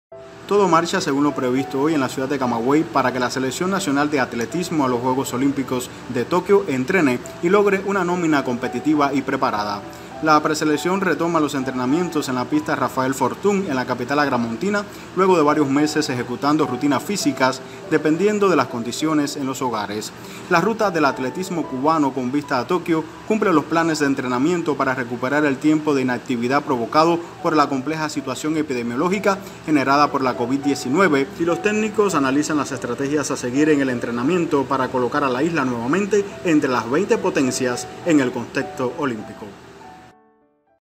Desde Camagüey entrena atletismo cubano para Juegos Olímpicos de Tokio
Todo marcha según lo previsto hoy en la ciudad de Camagüey  para que la selección nacional de atletismo a los Juegos Olímpicos de Tokio entrene  y logré una nómina competitiva y preparada.